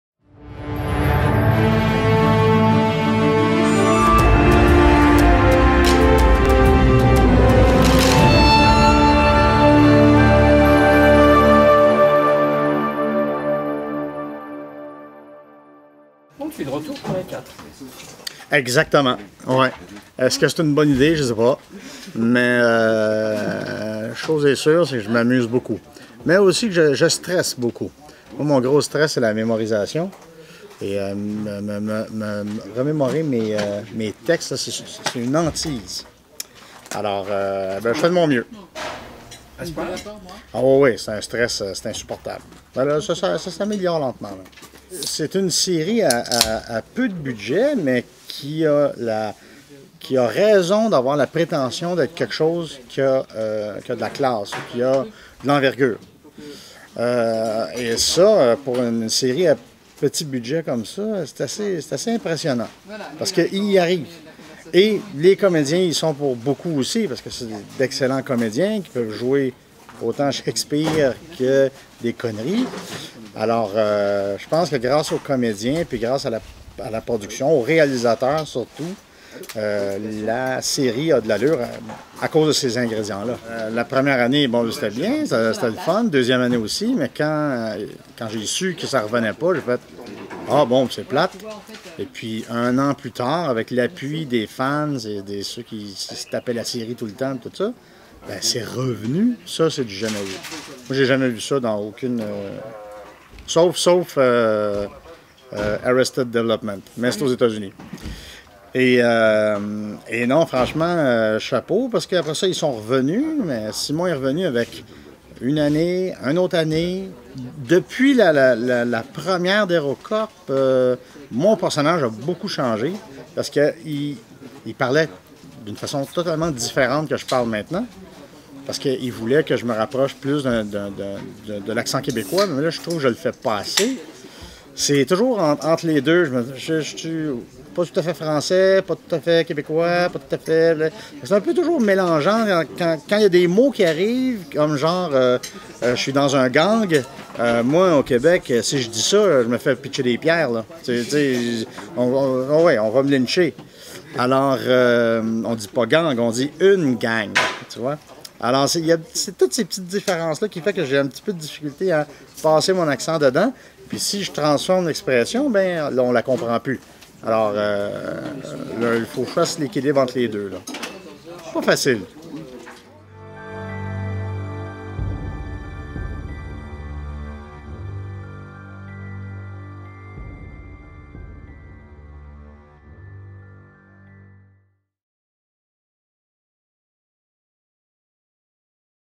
Hero Corp saison 4 - Courtemanche sur le tournage
Interview de Michel Courtemanche, alias "Captain Canada", sur le tournage de la saison 4 de "Hero Corp" (prochainement sur France 4).